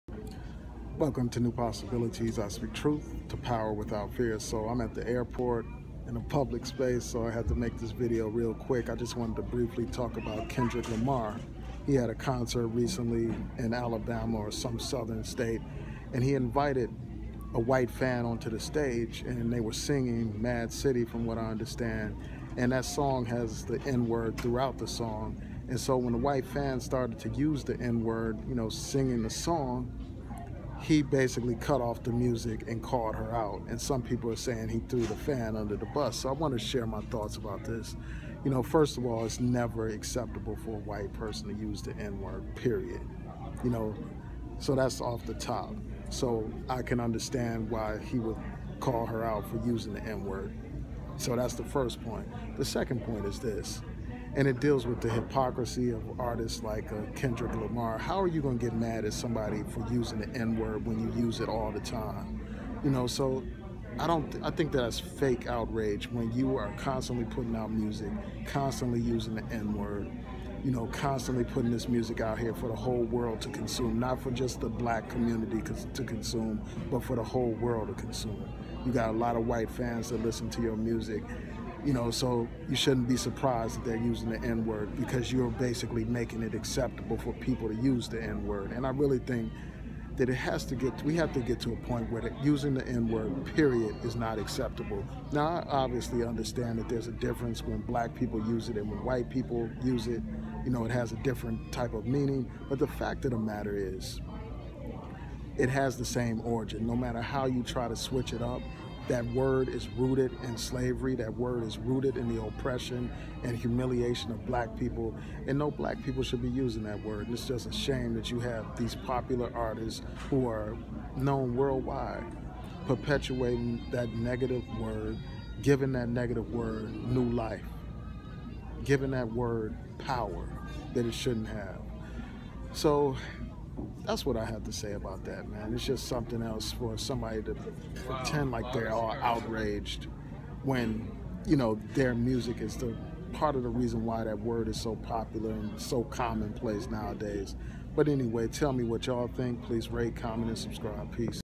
Kendrick Lamar, the White Fan and The N Word
Kendrick Lamar Calls Out a white fan for using the N word, Kendrick Lamar throws a white fan under the bus, white people and the N word, hip hop and the N word, the N word